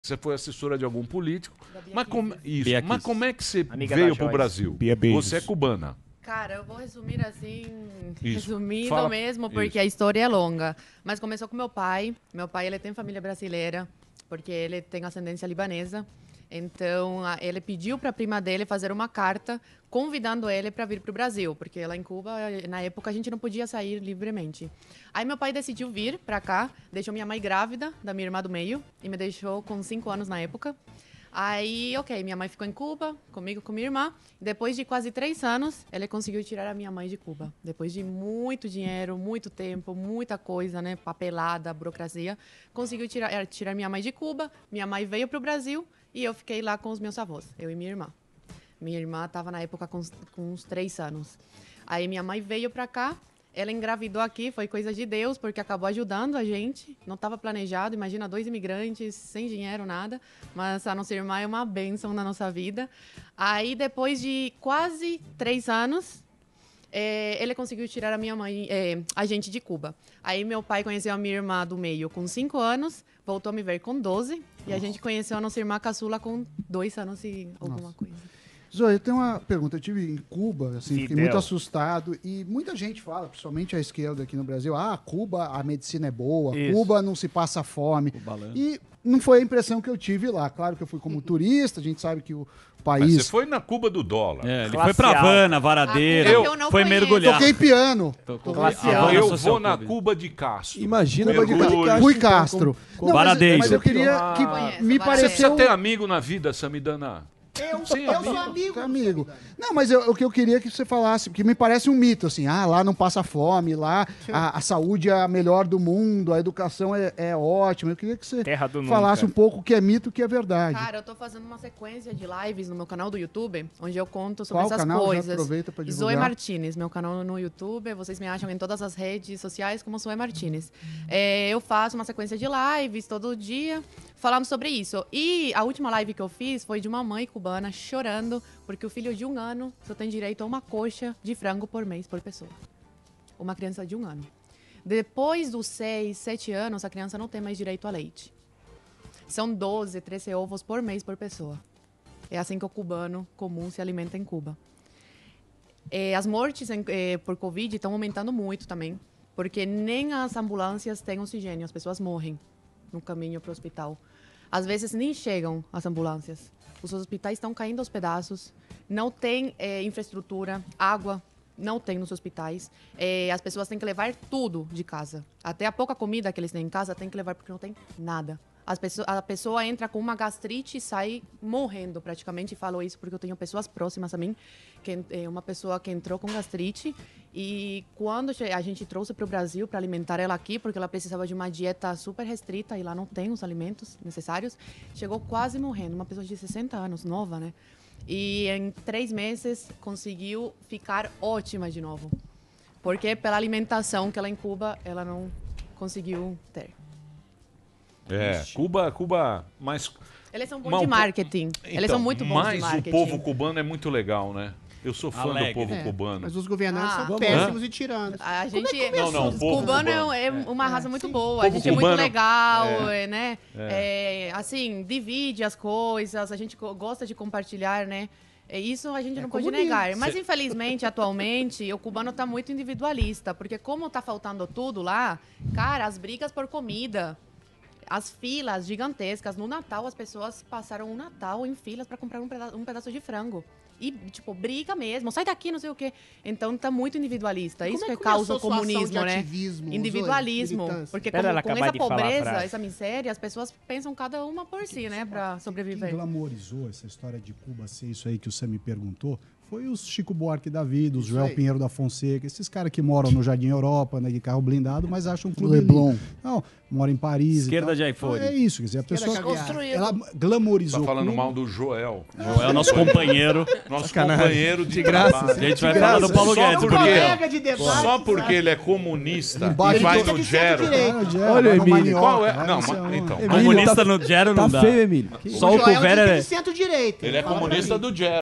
Zoe Martinez: 'INFELIZMENTE O CUBANO ESTÁ MUITO INDIVIDUALISTA, E ISSO QUE CAUSA O COMUNISMO'
Zoe Martinez conta como ela e sua família vieram para o Brasil, e fala sobre as lives que faz em seu canal no Youtube para contar às pessoas sobre a verdadeira realidade de Cuba e os problemas de saúde e alimentação